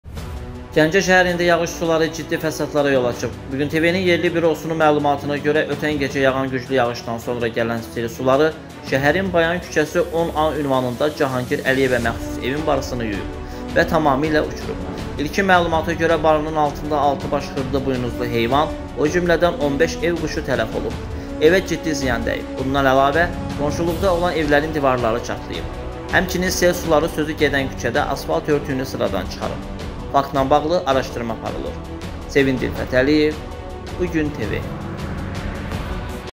Gəncədə sel suları fəsada yol açdı
Gəncə şəhərində yağış suları ciddi fəsadlara yol açıb.
Bugün Tv-nin yerli bürosunun məlumatına görə,ötən gecə yağan güclü yağışdan sonra gələn sel suları şəhərin Bayan küçəsi 10 a ünvanında Cahangir Əliyevə məxsus evin barısını yuyub və barı tamamilə uçub.
İlkin məlumata görə, barının altında 6 baş xırda buynuzlu heyvan,o cümlədən 15 ev quşu tələf olub. Evə ciddi ziyan dəyib.Bundan əlavə qonşuluqdakı evlərin divarları çatlayıb.Həmçinin sel suları sözü gedən küçədə   asfalt örtüyünün sıradan çıxardıb.